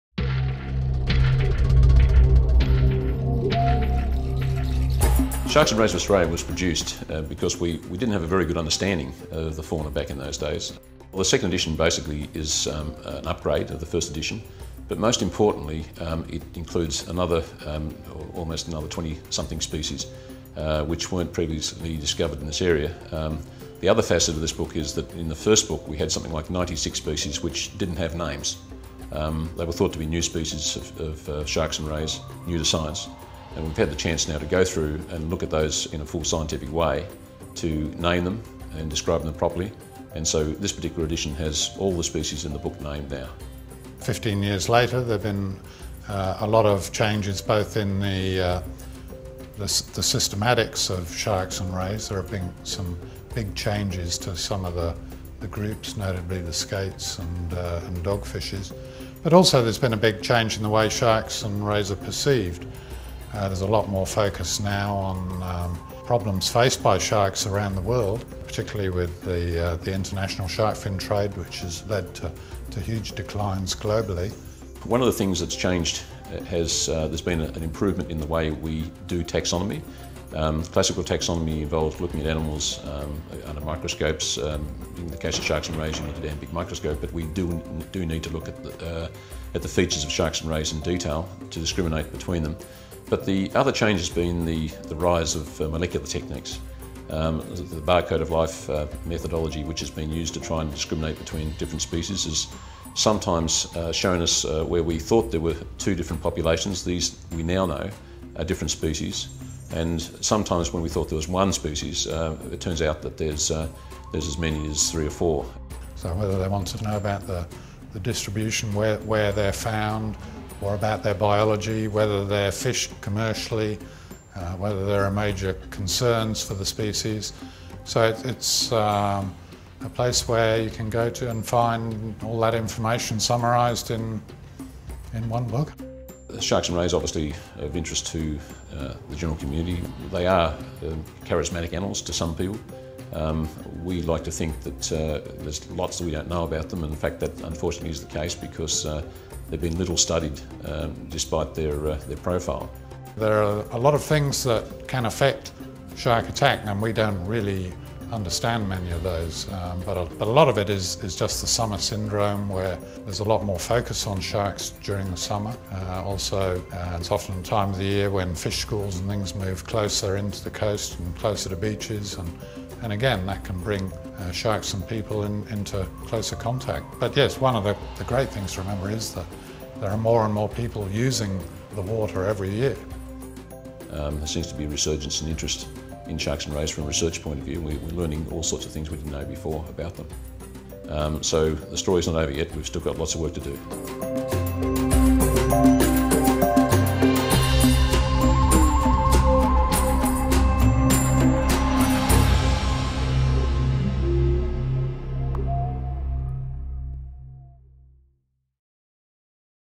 Sharks and Rays of Australia
The waters around Australia are home to the greatest diversity of sharks and rays on Earth. Spookfish, numbfish, stingarees, fiddler rays and cookie-cutter sharks are just some of the 322 shark, ray and chimaerid species illustrated in the magnificent new edition of Sharks and Rays of Australia.